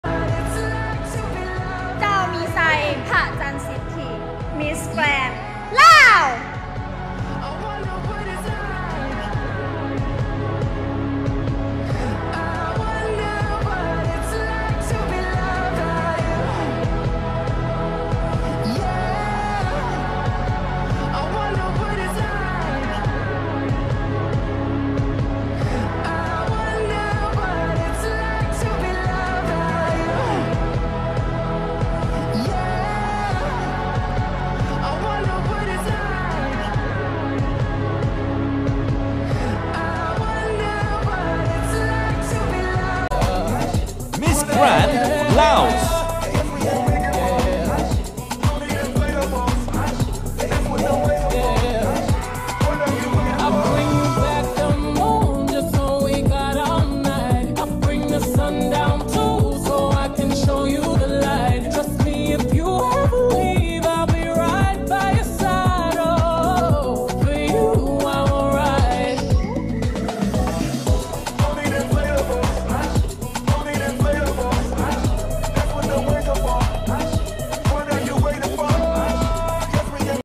Miss Grand Laos Laos | Preliminary - Miss Grand International 2021 | Full highlight
.- Evening gown .- MGI Swimsuit
.- Miss Grand Laos 2021, Dai Mixay Phachansitthi